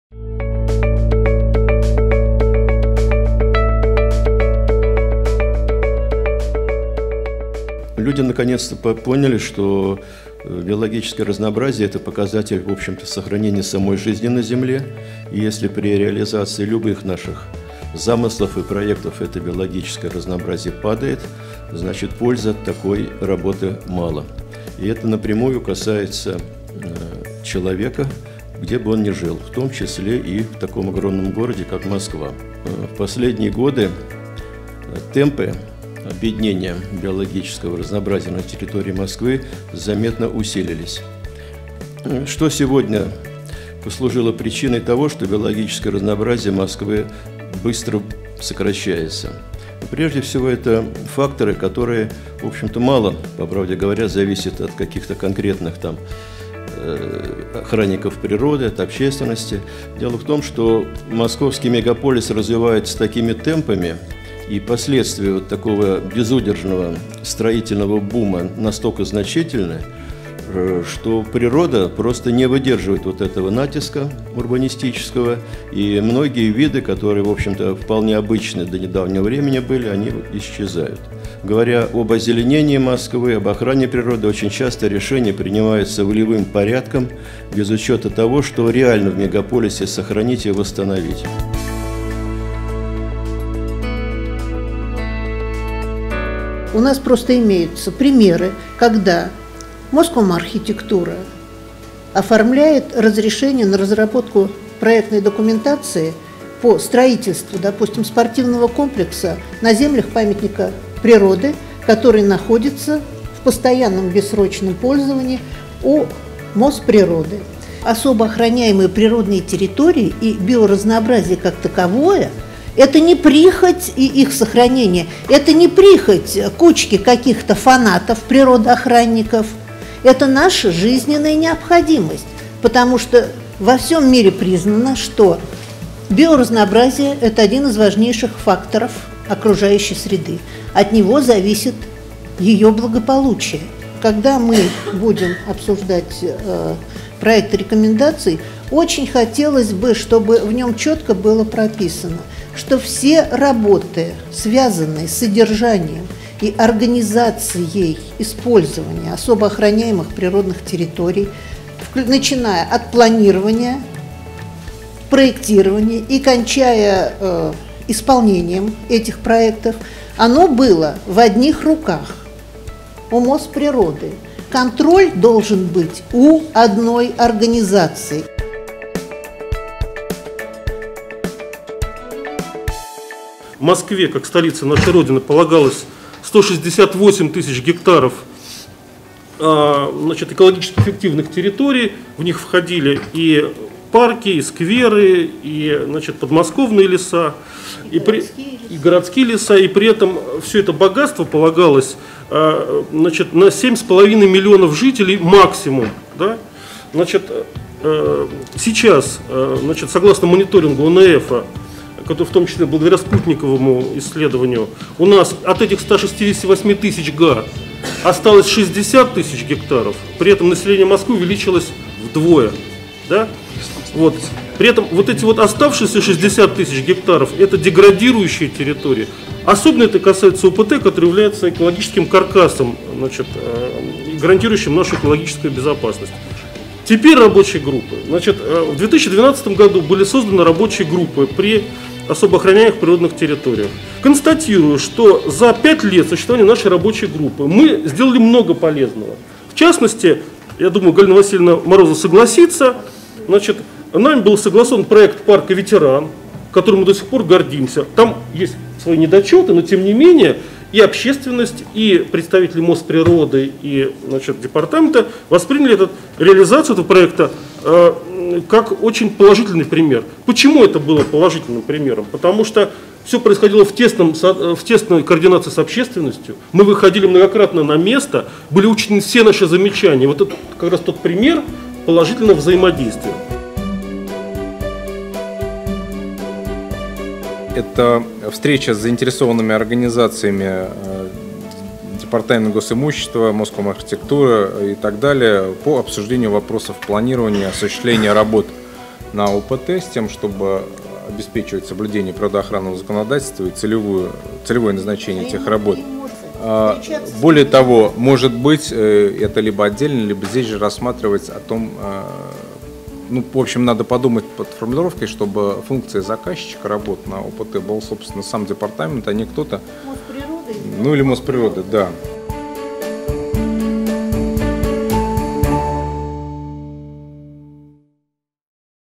МГФ-2017. Особо охраняемые и другие природные территории. Озеленение
О том, почему нужно заниматься грамотным озеленением? Зачем в мегаполисе нужно биоразнообразие? От кого защищают особо охраняемые природные территории? Ответы на эти другие вопросы ищите в видеоверсии главных тезисов дискуссии VIII Московского гражданского форума